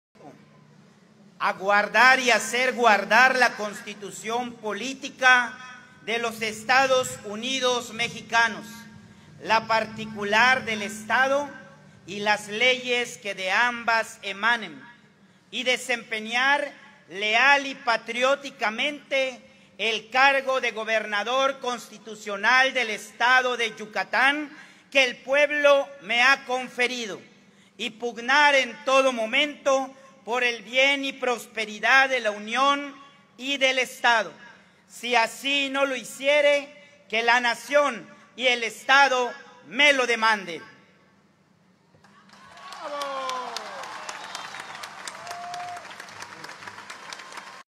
ASÍ RINDIÓ PROTESTA HUACHO DÍAZ COMO GOBERNADOR DE YUCATÁN
🔴 ENTÉRATE: ASÍ RINDIÓ PROTESTA HUACHO DÍAZ COMO GOBERNADOR DE YUCATÁN 📰🗞️